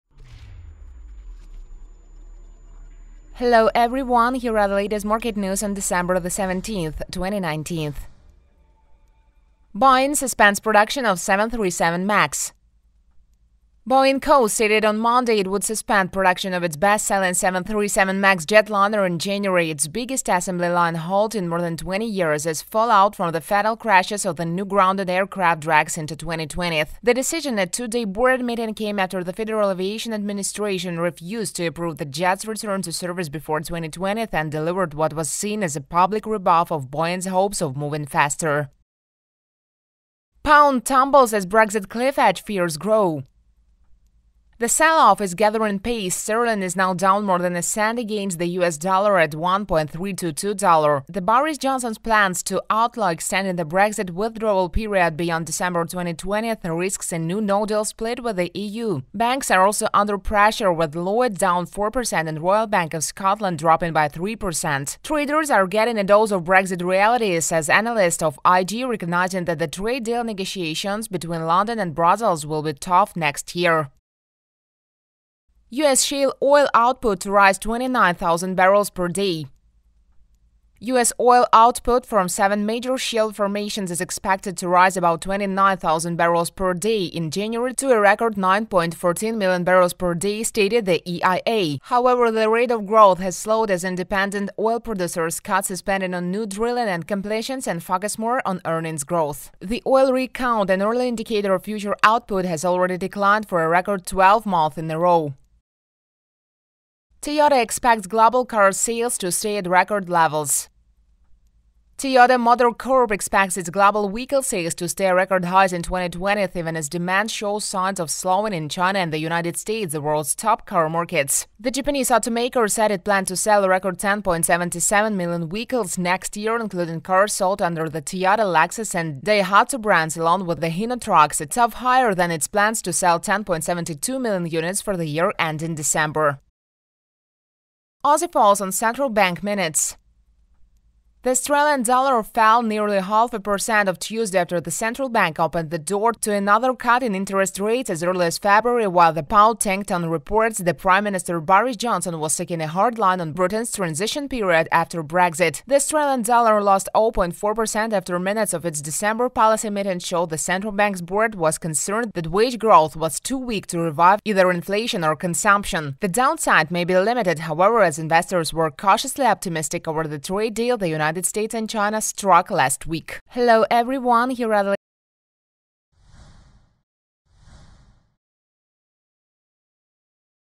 LBLV Boeing suspends production of 737 MAX 2019/17/12
LBLV provides an overview of economic news.

The main economic news for Tuesday, December 17:

1. Boeing suspends production of 737 MAX
Boeing Co stated on Monday it would suspend production of its best-selling 737 MAX jetliner in January, its biggest assembly-line halt in more than 20 years, as fallout from two fatal crashes of the now-grounded aircraft drags into 2020. The decision at a two-day board meeting came after the Federal Aviation Administration (FAA) refused to approve the jet’s return to service before 2020 and delivered what was seen as a public rebuff to Boeing’s hopes of moving faster.

2. Pound tumbles as Brexit cliff-edge fears grow
The selloff is gathering pace; Sterling is now down more than a cent against the US dollar at $1.322 Boris Johnson’s plan to outlaw extending the Brexit withdrawal period beyond December 2020 risks a new no-deal split with the EU. Banks are also under pressure, with Lloyds down 4% and Royal Bank of Scotland dropping by 3%. Traders are getting a dose of Brexit reality, says analyst of IG, recognizing that the trade deal negotiations between London and Brussels will be tough next year.

3. U.S. shale oil output to rise 29,000 bpd
U.S. oil output from seven major shale formations is expected to rise about 29,000 barrels per day (bpd) in January to a record 9.14 million bpd, stated the EIA.  However, the rate of growth has slowed as independent oil producers cut spending on new drilling and completions and focus more on earnings growth. The oil rig count, an early indicator of future output, has already declined for a record 12 months in a row.

4. Toyota expects global car sales to stay at record levels
Toyota Motor Corp expects its global vehicle sales to stay at record highs in 2020, even as demand shows signs of slowing in China and the United States, the world’s top car markets. The Japanese automaker said it planned to sell a record 10.77 million vehicles next year, including cars sold under the Toyota, Lexus and Daihatsu brands along with Hino trucks, a touch higher than its plans to sell 10.72 million units for the year ending December.

5. Aussie falls on central bank minutes
The Australian dollar fell nearly half a percent on Tuesday after the central bank opened the door to another cut in interest rates as early as February while the pound tanked on reports that Prime Minister Boris Johnson was seeking a hard line on Britain’s transition period after Brexit. The Australian dollar lost 0.4% after minutes of its December policy meeting showed the central bank's board was concerned that wage growth was too weak to revive either inflation or consumption. The downside may be limited, however, as investors were cautiously optimistic over the trade deal the United States and China struck last week.